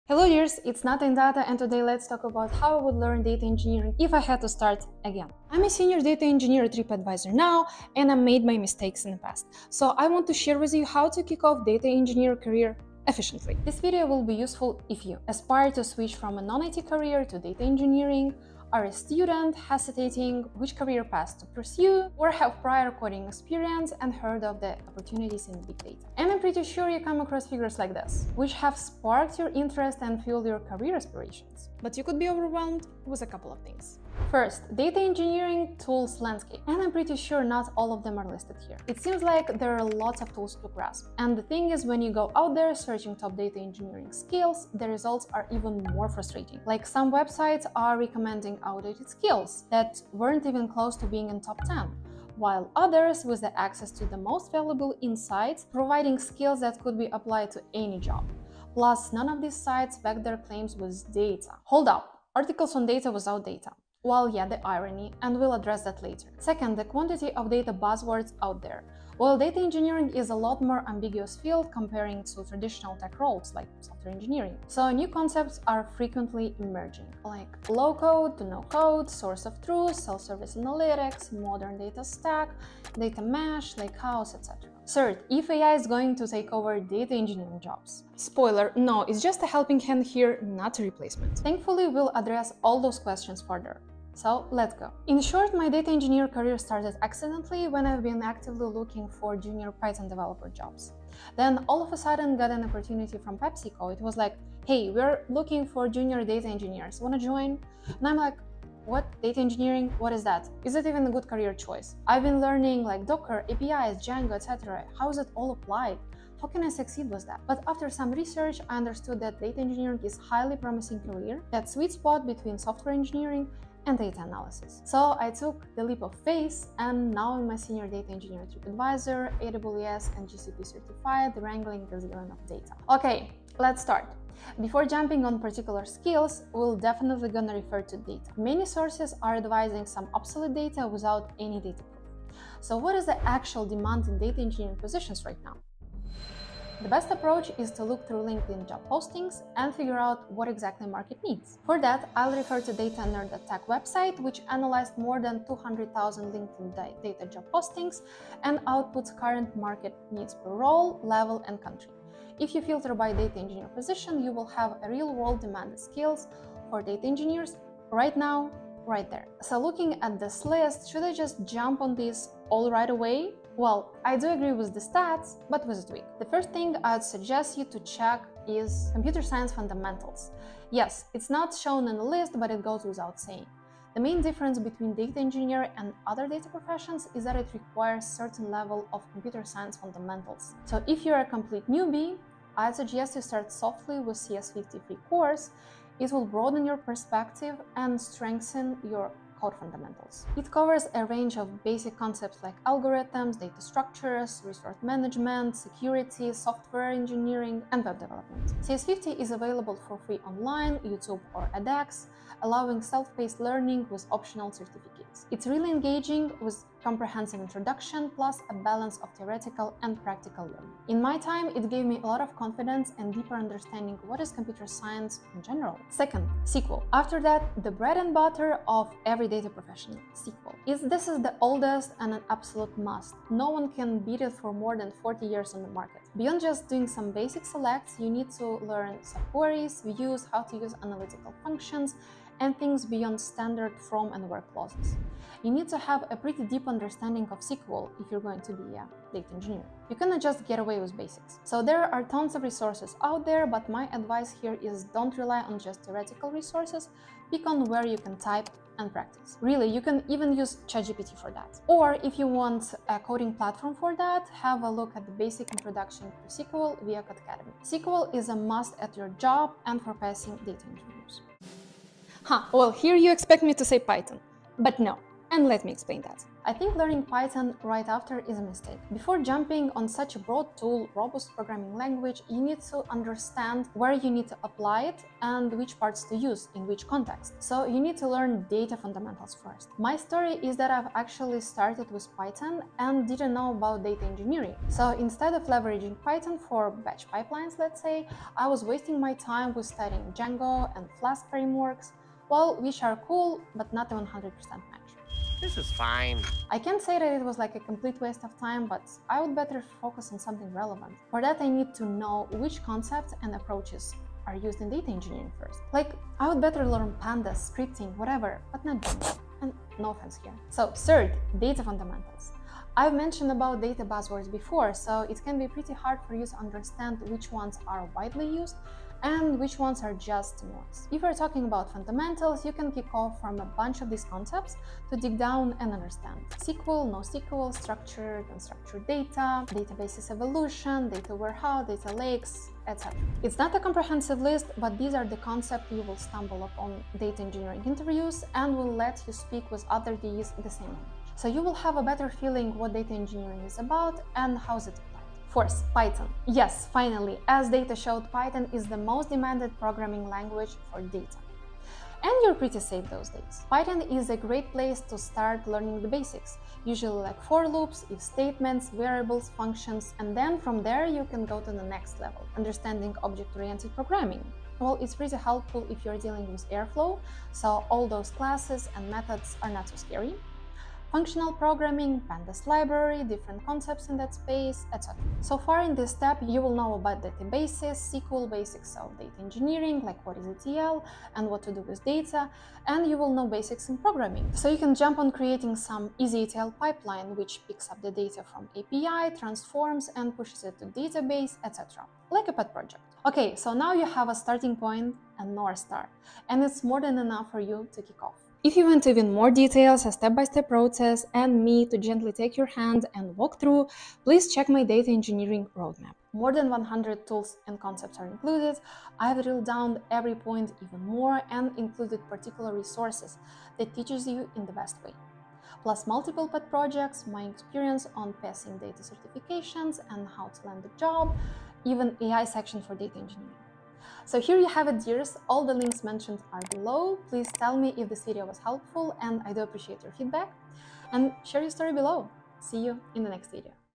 How I would learn Data Engineering if I had to start AGAIN 2024
Data Engineering Roadmap 2024: How to Become a Data Engineer

🏅 COURSES & CERTIFICATES

TIMESTAMPS:

0:00 Intro

0:32 Data Engineering Frustrations

01:35 My career path

02:13 Real Demand For Data Engineers

02:58 Computer Science Fundamentals

03:46 SQL

04:31 ...🥁

05:21 Data Fundamentals

05:55 Python

06:48 Complete Data Engineering Roadmap

Please tell me if this video was helpful, I do appreciate your feedback!

Share your story below ⬇️